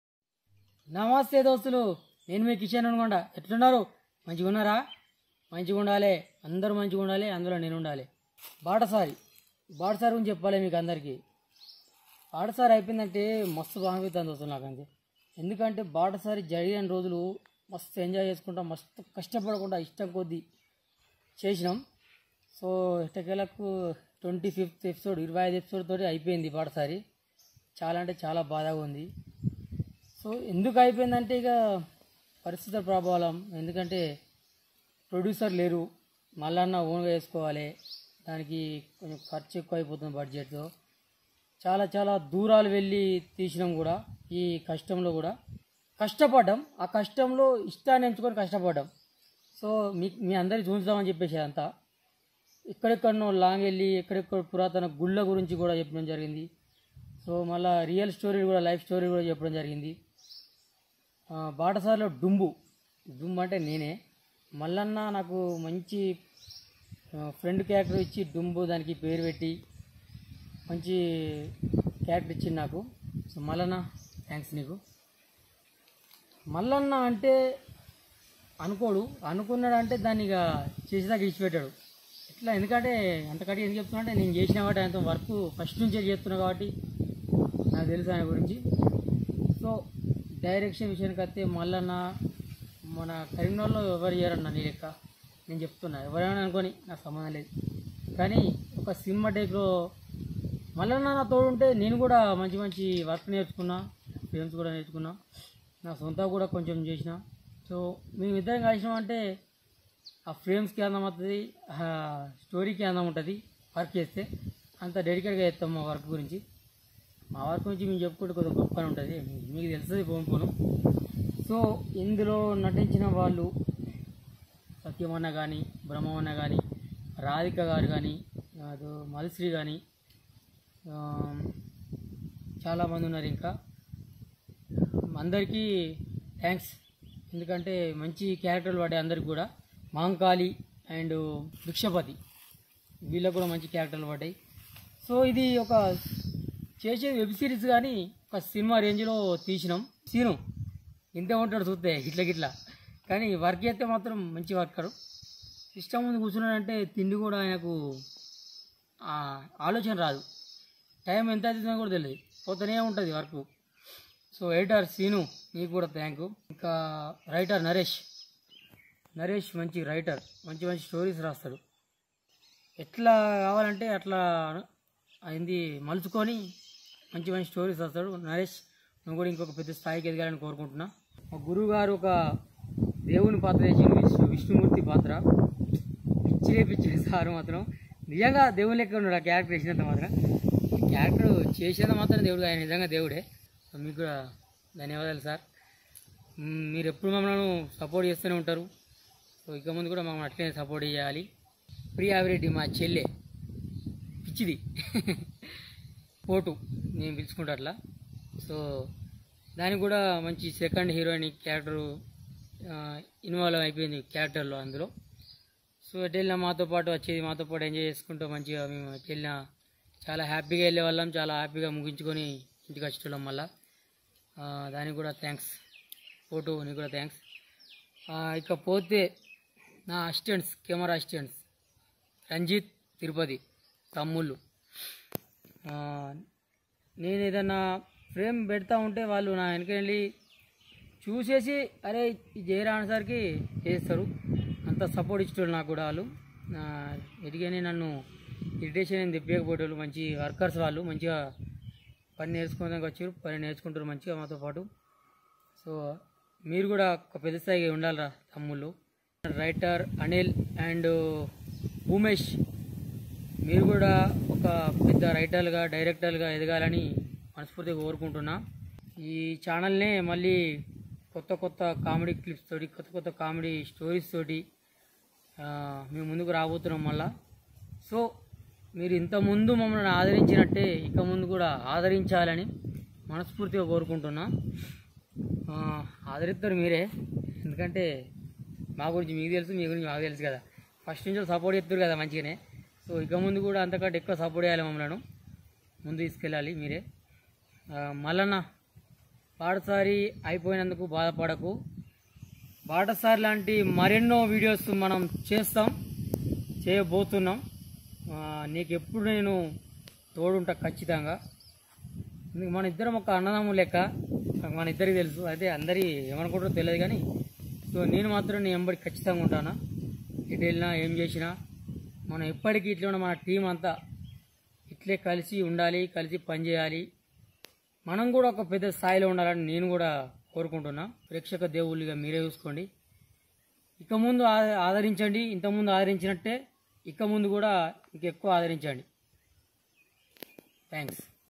Kishan Nunugonda Talk About Batasari | Batasari Web Series 2021
In this video, He has been told very well about his experience and about the batasari.